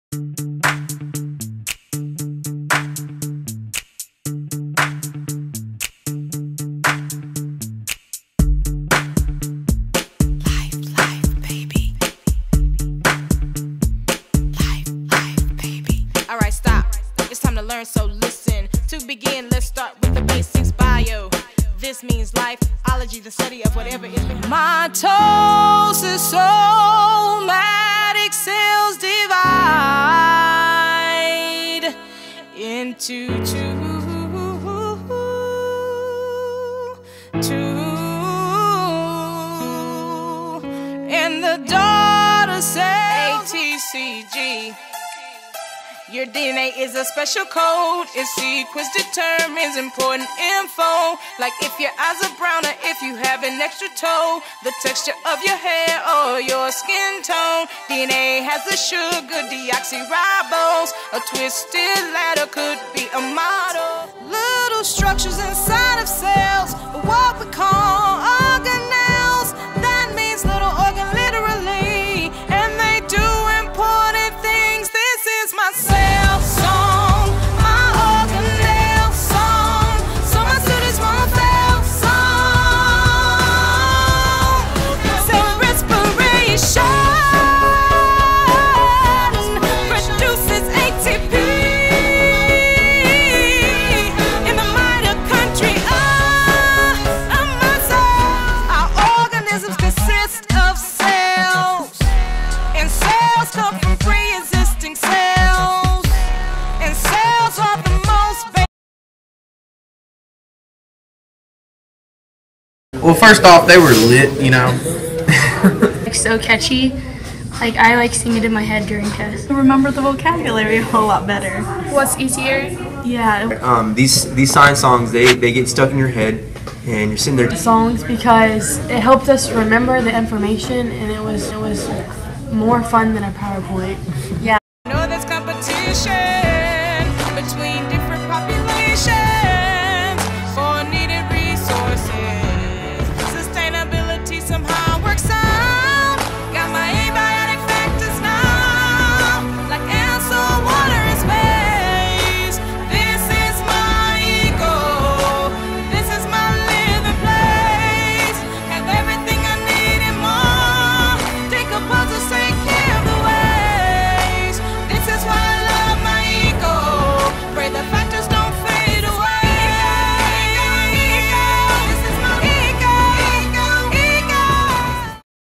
Science Songs
I have finally done it.  Science Songs are now available.  They are professionally recorded.  HELP ME SPREAD THE WORD.  Do you know a science teacher, principal, assistant principal, child going into the 6th, 7th, 8th or 9th grade?  If you do please share this with them.  Science Songs Work! (and they are HOT!) lol